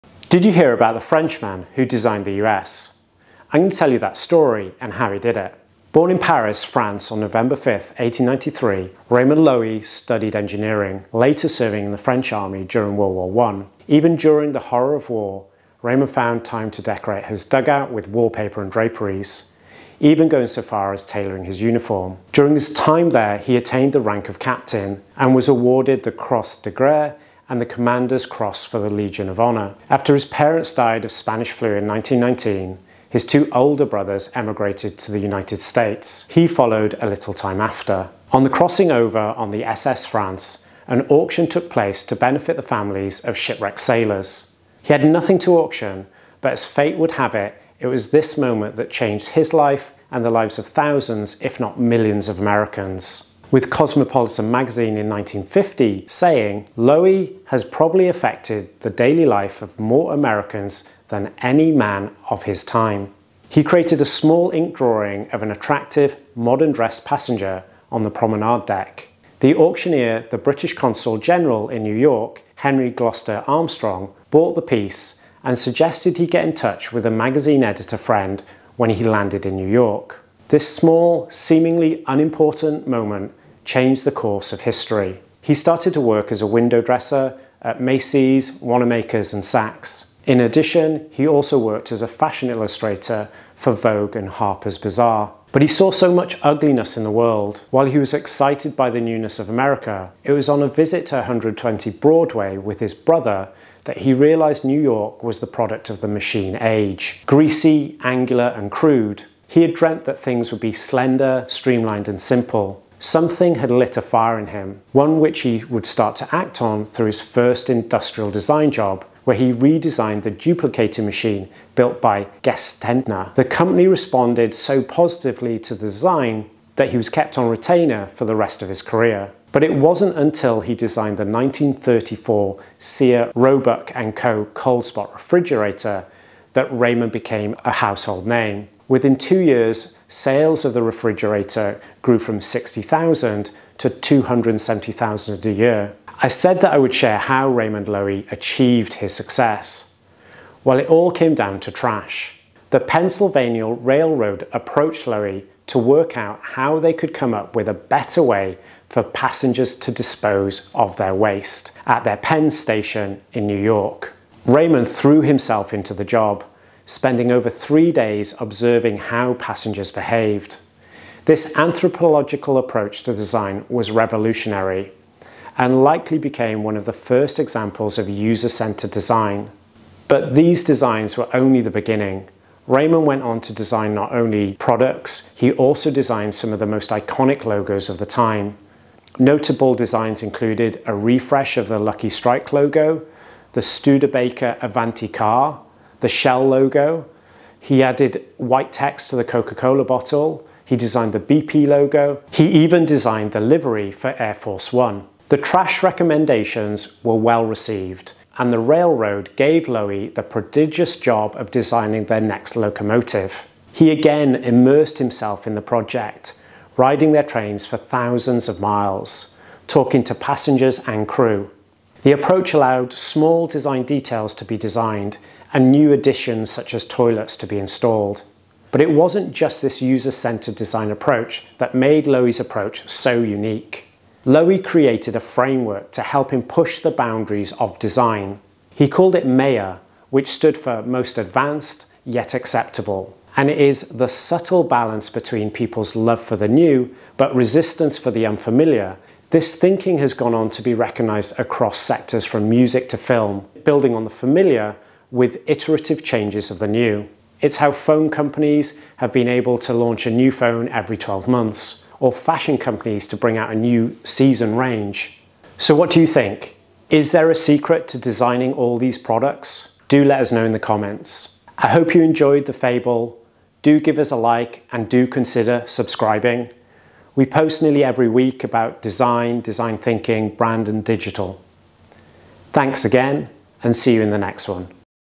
The man who designed the USA, Raymond Loewy.
We share the story of Raymond Loewy. One of the most well respected designers of the early 1900's.

Others are licensed through Unsplash
All other images owned by Hoffi

0:00 The life of Raymond Loewy
3:58 The revolutionary framework MAYA